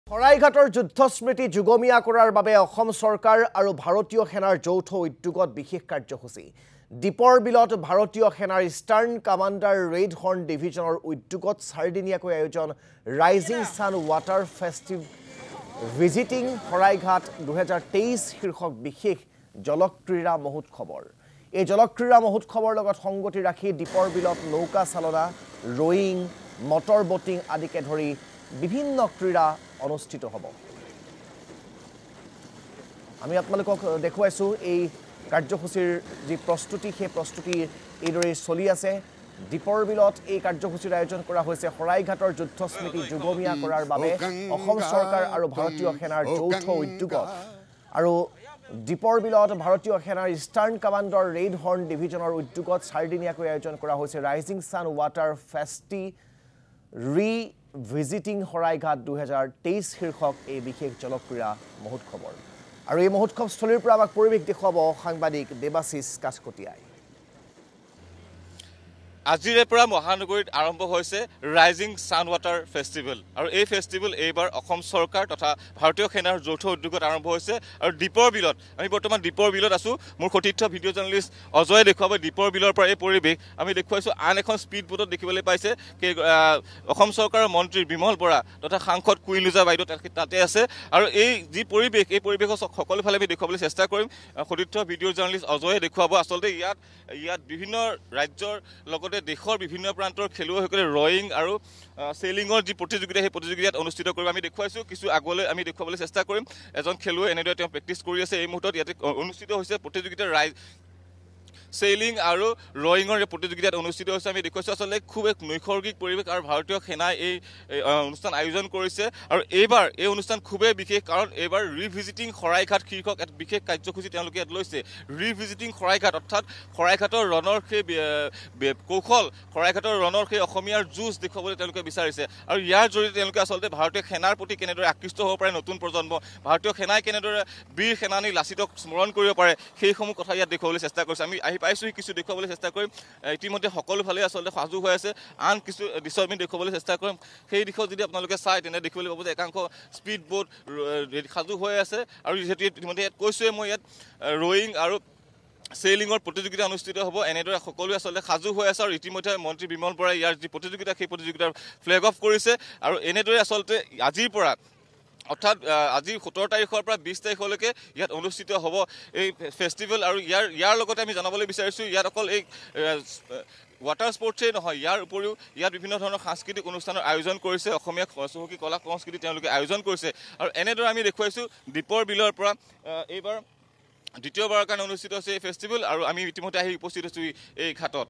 ৰাইজিং ছানৰ ৱাটাৰ ফেষ্ট-ৰি ভিজিটিং শৰাইঘাট-২০২৩
অসম চৰকাৰ, ভাৰতীয় সেনাৰ যৌথ উদ্যোগত বিশেষ কাৰ্যসূচী। ৰাইজিং ছানৰ ৱাটাৰ ফেষ্ট-ৰি ভিজিটিং শৰাইঘাট-২০২৩। চাৰিদিনীয়াকৈ আয়োজন এই বিশেষ জলক্ৰীড়া মহোৎসৱৰ। শৰাইঘাটৰ যুদ্ধ স্মৃতি যুগমীয়া কৰাৰ উদ্দেশ্যে এই বিশেষ কাৰ্যসূচী।